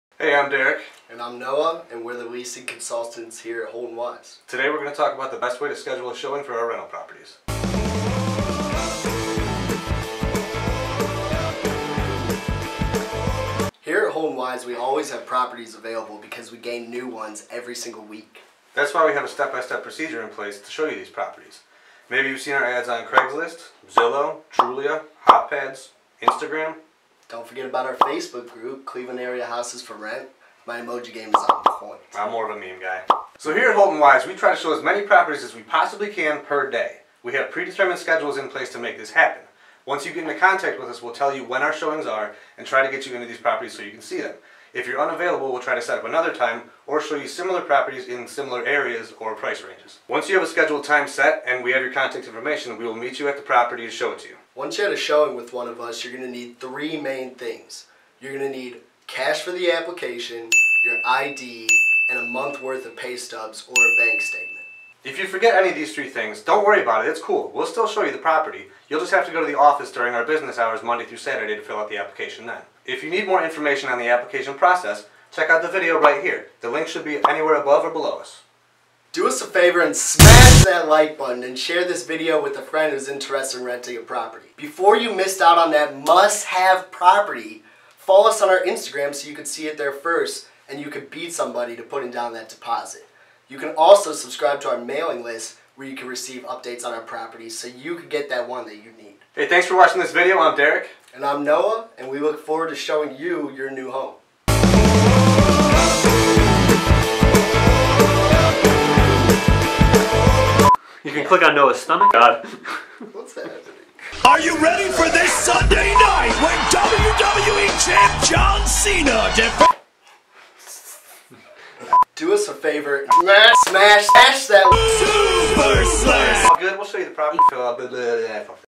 Cleveland Property Management: Scheduling a Tour of Houses For Rent | HoltonWise FAQ
In this video HoltonWise Leasing Consultants Derek and Noah go over the process of scheduling a showing/tour of Holton-Wise's rental houses, apartments or retail locations.